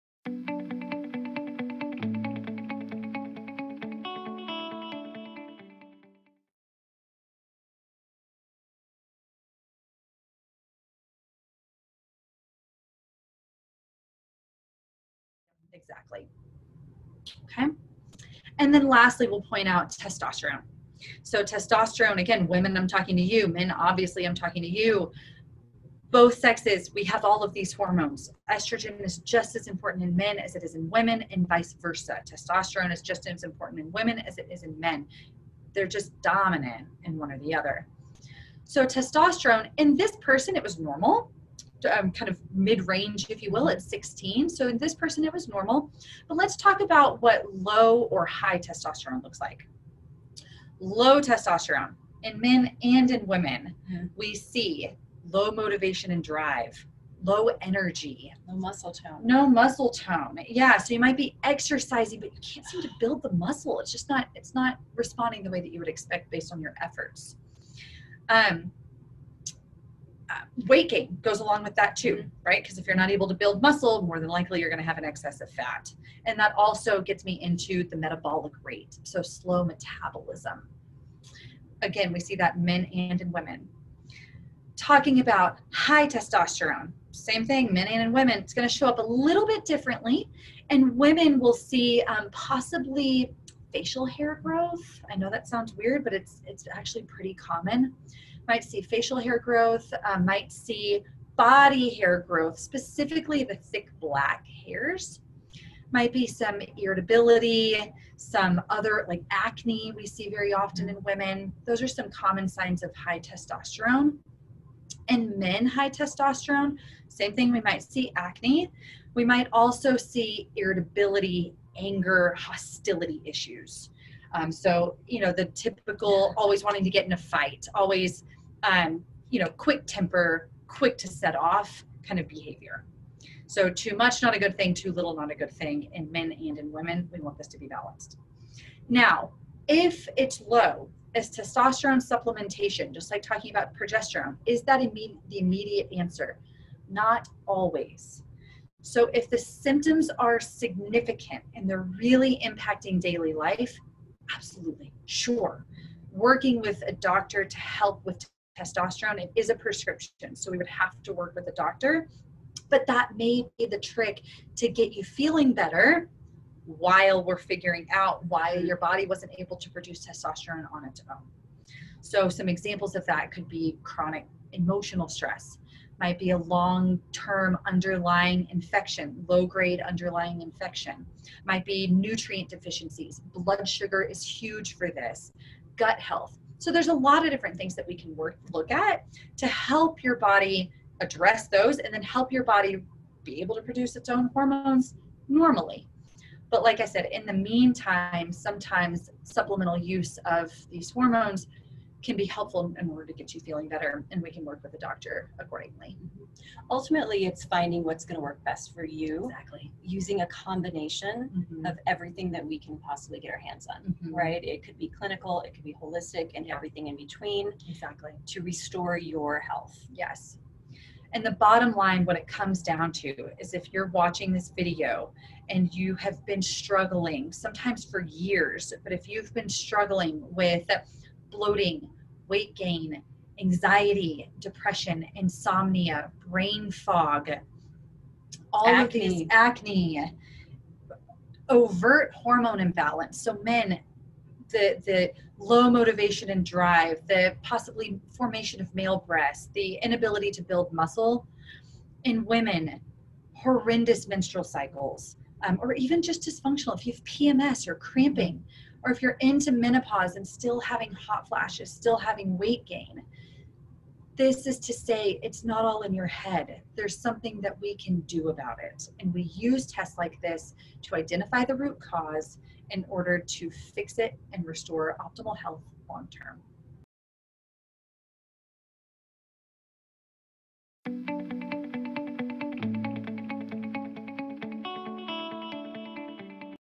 Hormones - Chapter 5
Why we offer Hormonal testing at Nutrition Works and how it can benefit those we help!

Want to know more and schedule your FREE discovery phone call to get your health back on track, go